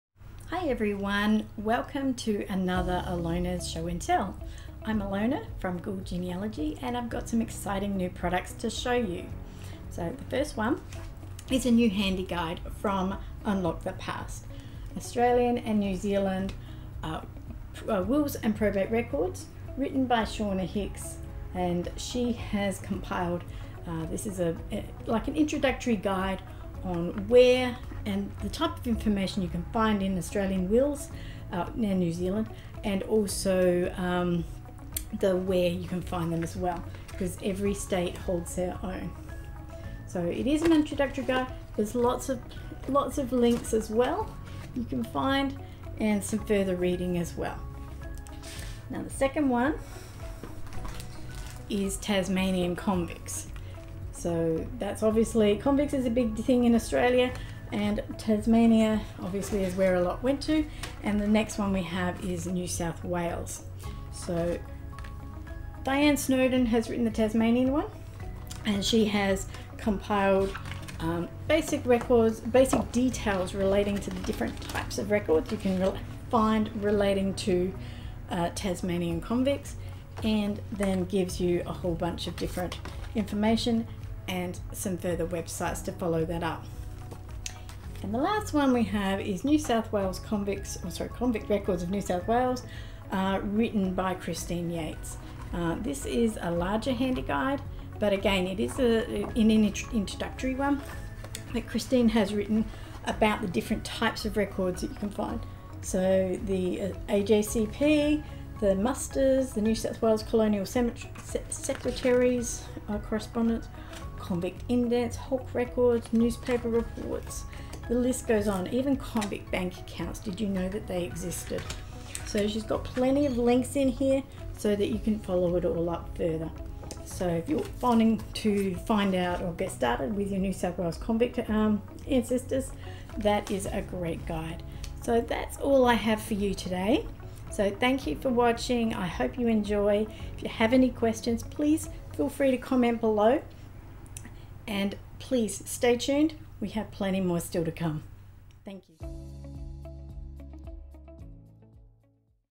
New guides to help research Australian convicts and wills
Unlock the Past have recently added to their range of great easy-to-read guides with the release of three new Handy Guides. Alona talks a little about these recent family history releases. Join her and learn a bit more about each one.

Handy Guide: Australian and New Zealand Wills and Probate Records

Handy Guide: Convict Records of New South Wales: The Human Stories of the Transportation System

00:00 Introduction
00:09 Australian and New Zealand Wills and Probate Records
00:48 Tasmanian Convicts
01:30 Convict Records of New South Wales
02:15 Conclusion